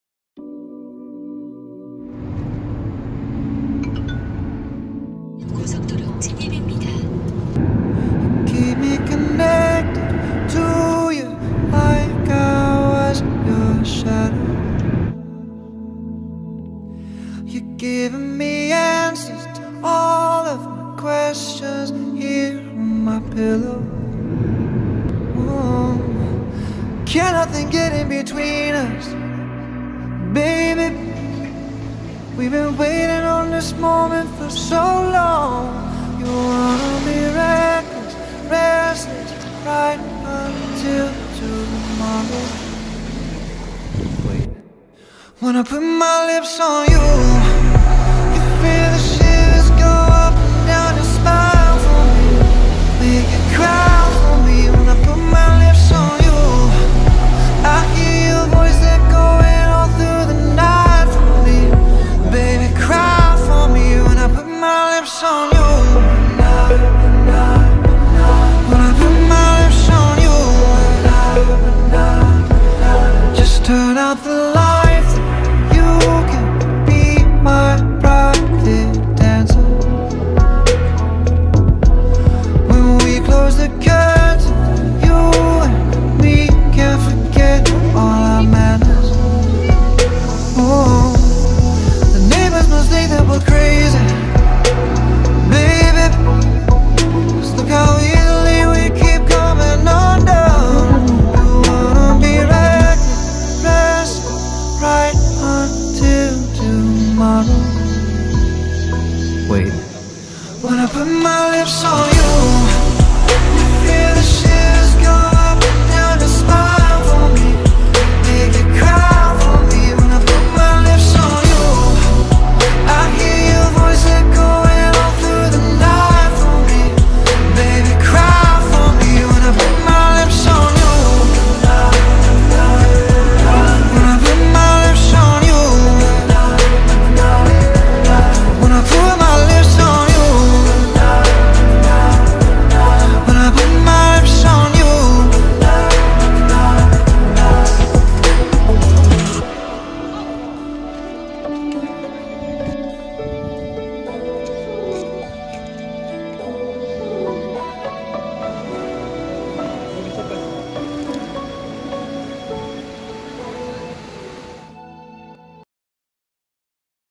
My 2018 Family Summer Holiday | Vlog
매년 가는 가족 여행으로, 올해는 강원도 고성을 갔다왔다.